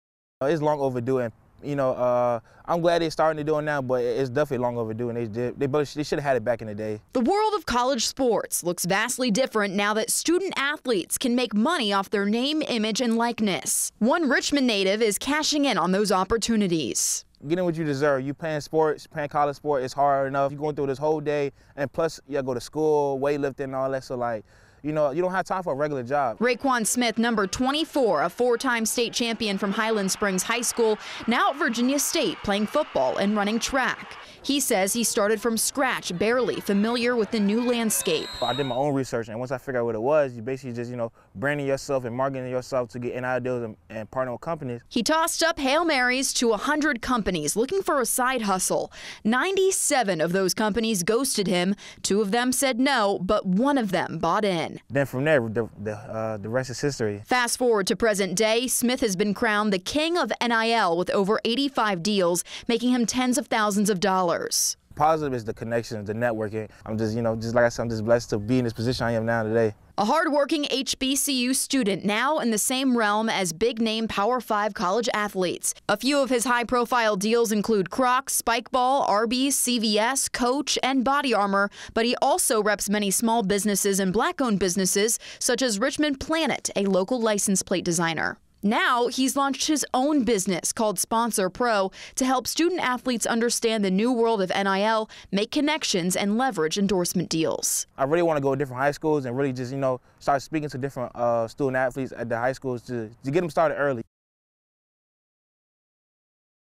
The Real King of NIL resides at a HBCU with over 100 NIL Deals
Name, image and likeness is now part of college athletics. Riley Wyant introduces us to Rayquan Smith who attends the HBCU, Virginia State where he plays both football and track. He has nearly 100 NIL deals. (Credit: WWBT via NBC)